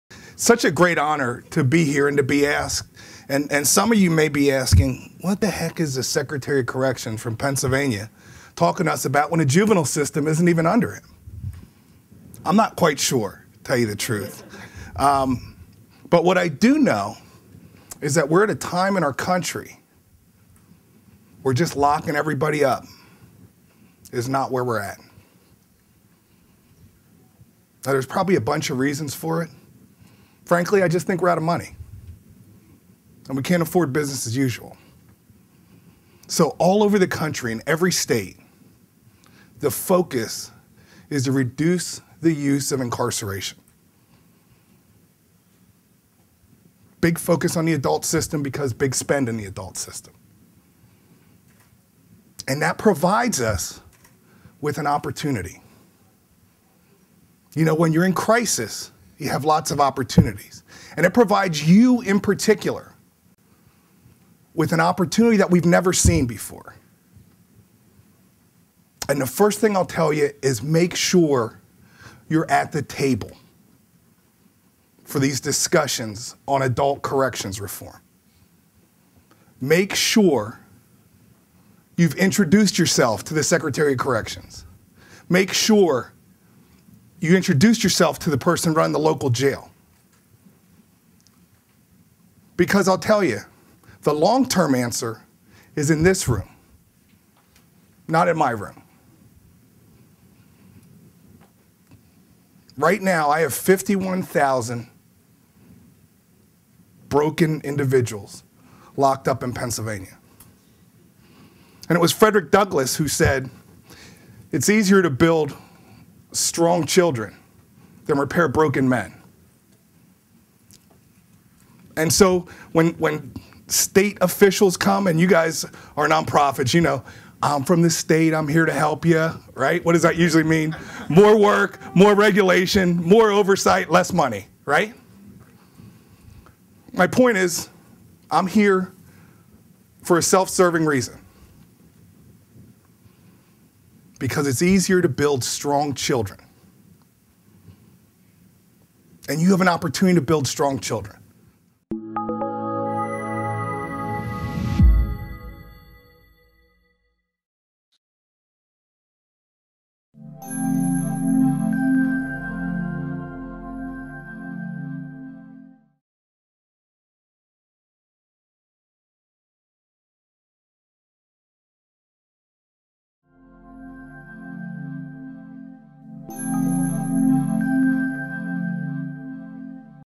John Wetzel: It's Easier to Build Strong Children
John Wetzel, Pennsylvania Secretary of Corrections, discusses the connection between the adult prison system and the youth service field.